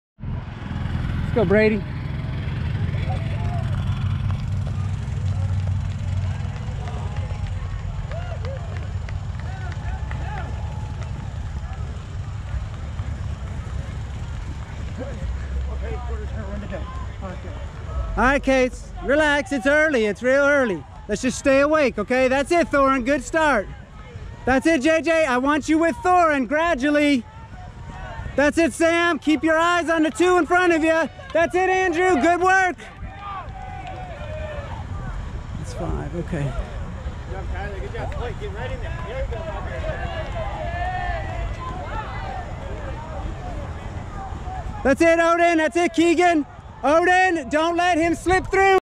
20210904 Memphis Twilight Boys HS 2
September 4, 2021 City Auto Memphis Twilight cross country meet at the Mike Rose Soccer Complex in Memphis, TN hosted by Christian Brothers University.